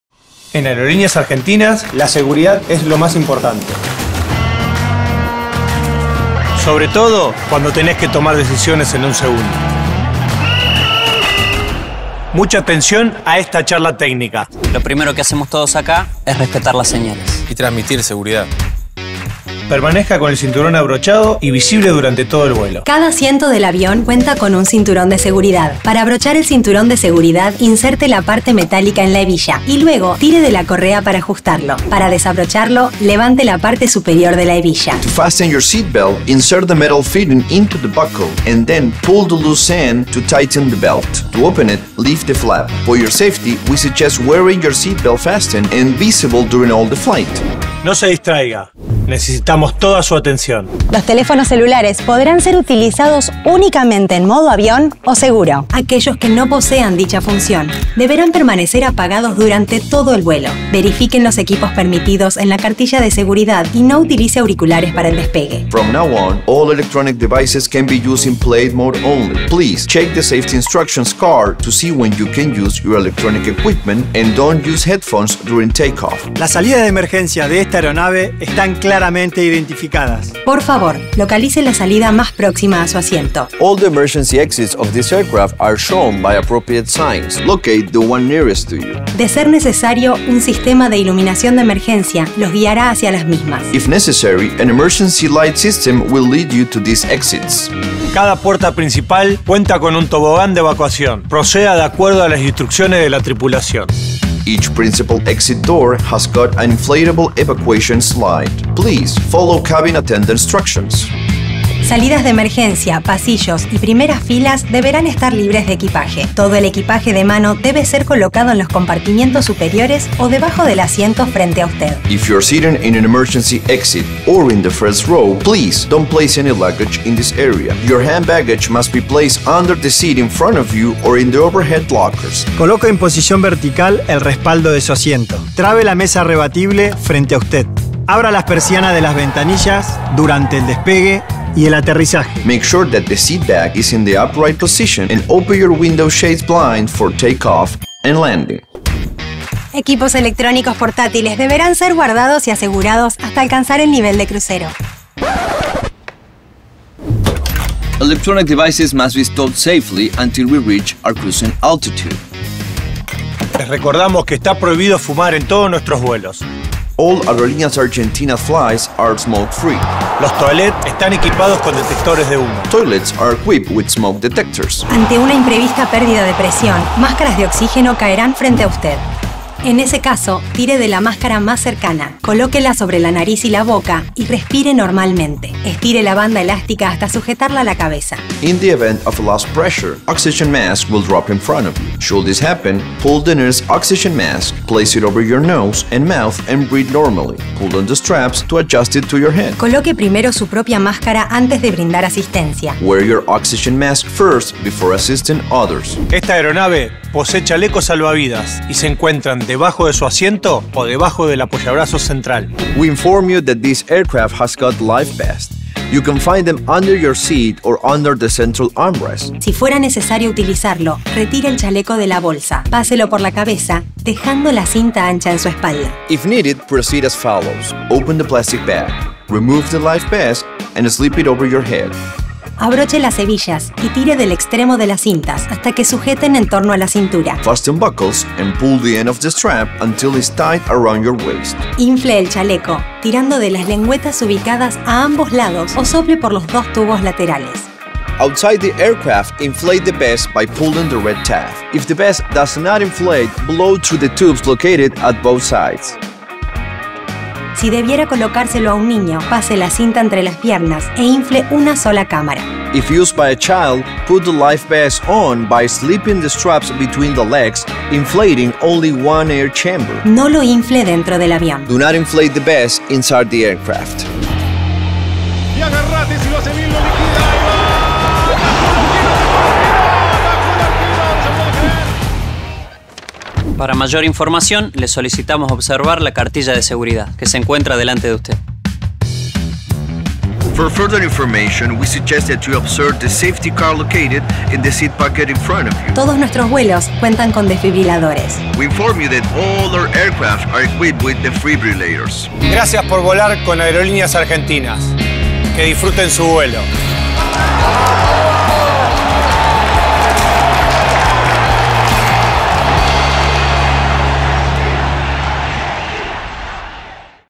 Aerolíneas Argentinas presentó su nuevo video de seguridad con el cuerpo técnico de la Selección 🇦🇷
Aerolíneas Argentinas presentó su nuevo video de seguridad con el cuerpo técnico de la Selección Argentina.

Lionel Scaloni, Pablo Aimar, Roberto Ayala y Walter Samuel son los protagonistas del video con las indicaciones de seguridad de la línea aérea. Fue presentado en el " C Complejo Art Media".